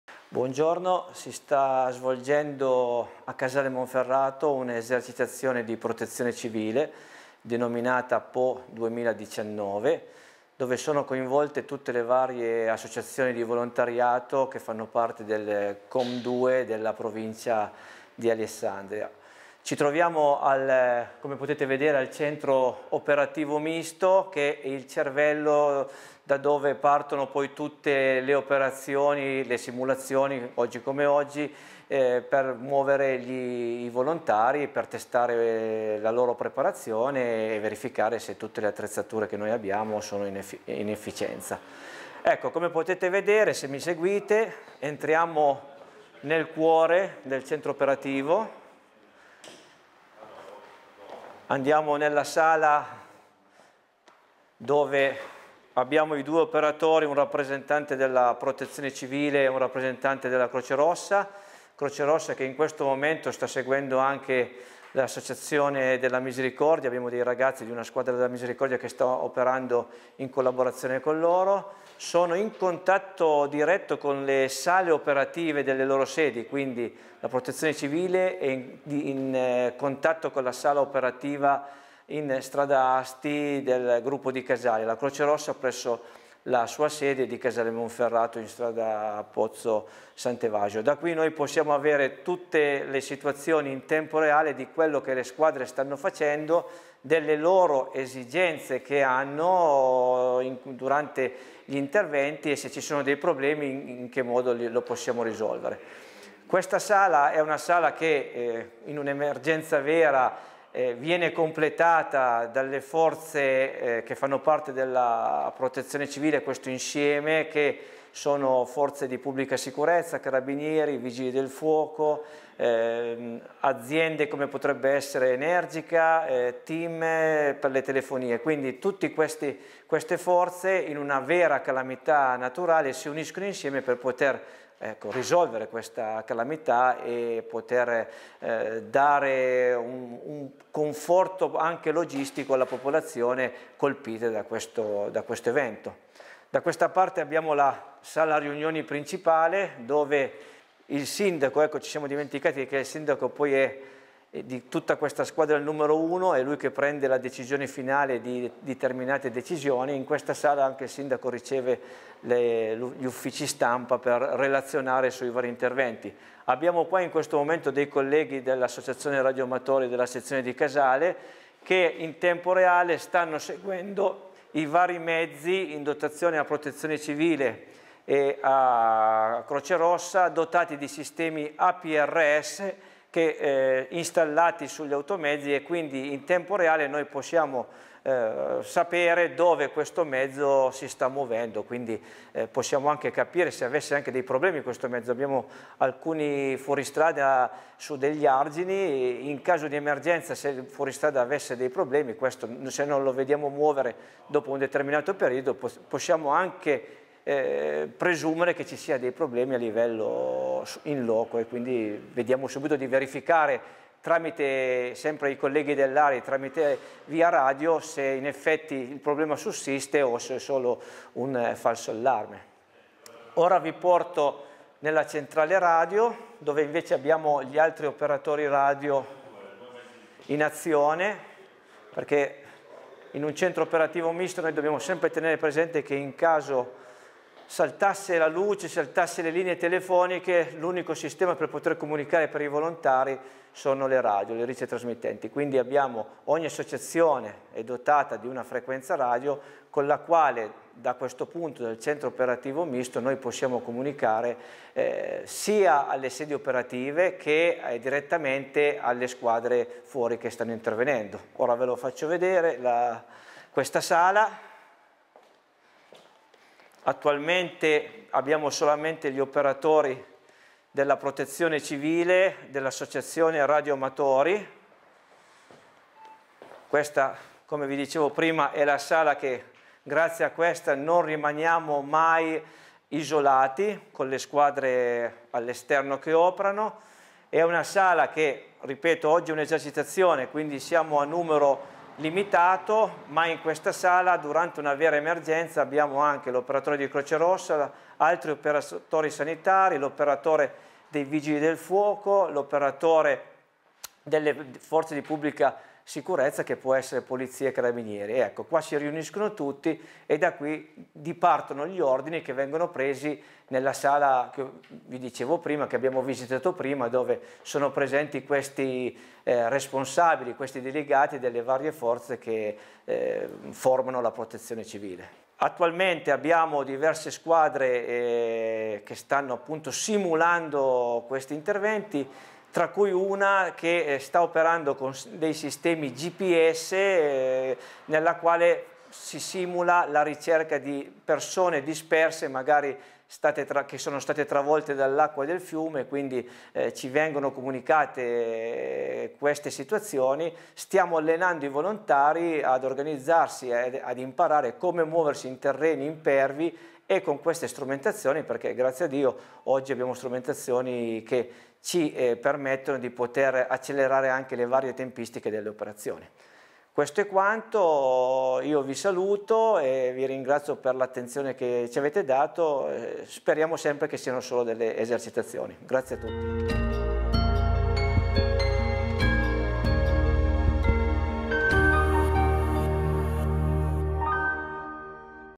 Protezione Civile: "Esercitazione PO 2019" - come è organizzato il COM 2 di Casale Monferrato
Si sono svolte sabato 19 ottobre a Casale Monferrato le annuali esercitazioni di Protezione Civile con le associazioni che fanno parte del COM 2 (Centro Operativo Misto) della Provincia di Alessandria.
Abbiamo avuto l'opportunità di visitare i locali che ospitano il COM,  punto di riferimento per il territorio Casalese, vedere come è organizzato e come dovrebbe funzionare durante le emergenze.